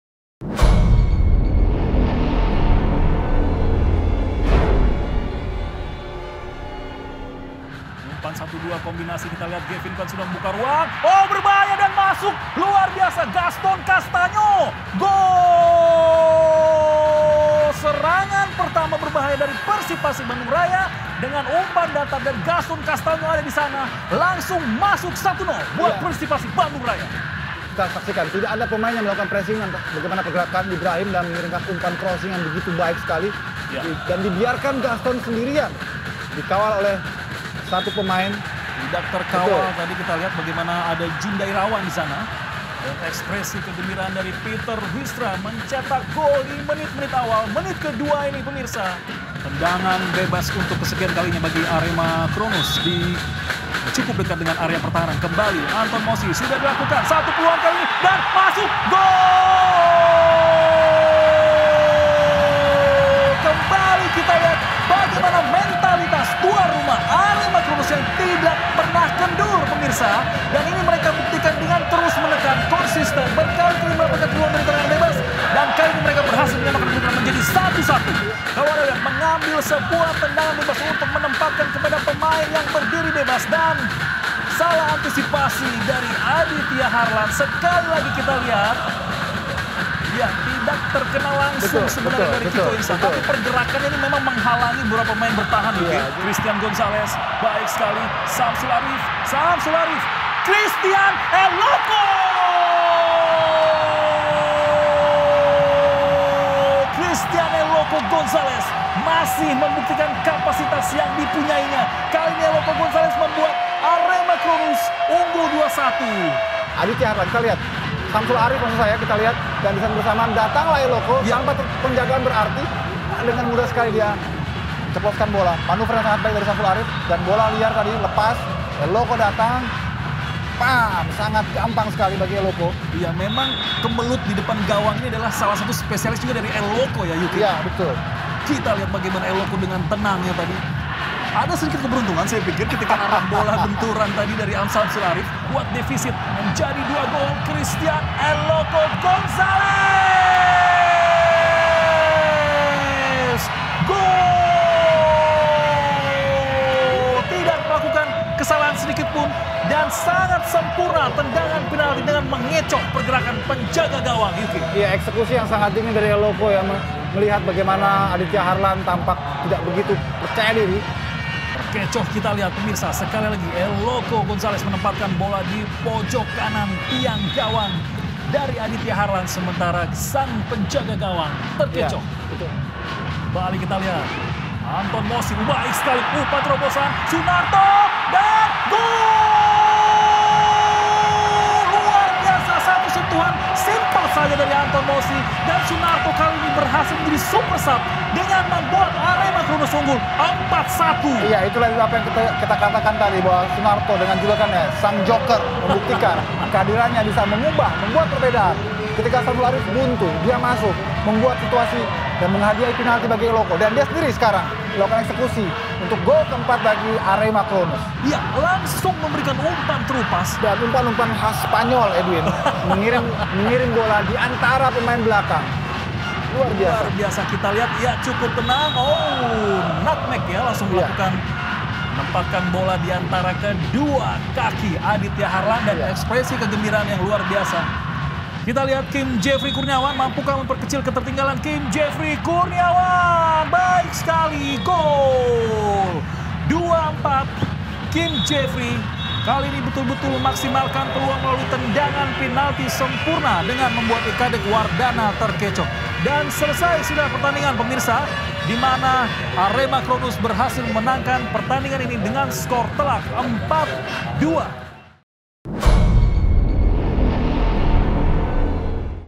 Grup A: Arema Cronus vs Persipasi Bandung Raya (4-2) - Match Highlights
Indonesian Championship Torabika 2015 memperebutkan Jenderal Sudirman Cup